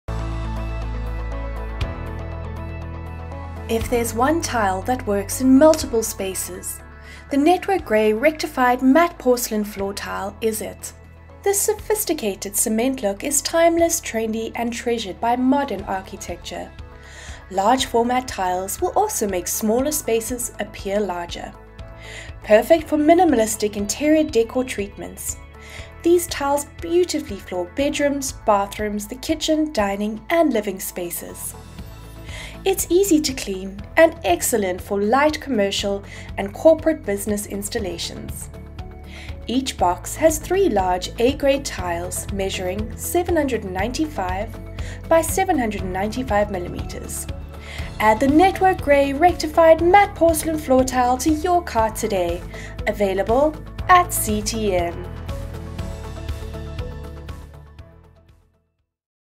Network Grey Rectified Matt Porcelain Floor Tile - GR1CNE20R12
If there’s one tile that works in multiple spaces the Network Grey Rectified Matt Porcelain Floor Tile is it!

This sophisticated cement look is timeless, trendy & treasured by modern architecture.

 Large-format tiles will also make smaller spaces appear larger.

 Perfect for minimalistic interior décor treatments, these tiles beautifully floor bedrooms, bathrooms, the kitchen, dining & living spaces.

 It’s easy to clean & excellent for light commercial and corporate business installations.

Each box has three large A-grade tiles measuring 795 x 795 millimeters. Exclusively available at CTM